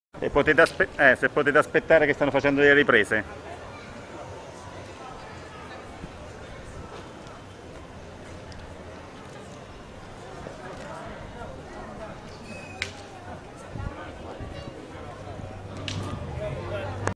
Porta Romana nel BackStage Giapponese
Backstage pubblicitario della Città di Ascoli per il mercato giapponese con la presenza degli Sbandieratori di Porta Romana ed uno degli attori giapponesi più famosi e carini.